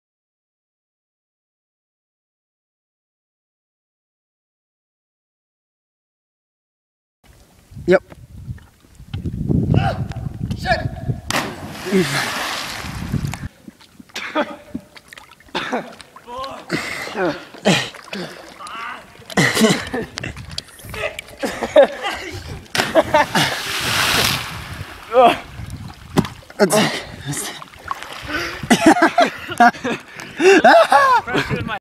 Barry's Landing Cliff Jumping
The new Bing Bong Bros enjoy a nice day cliff jumping out in Montana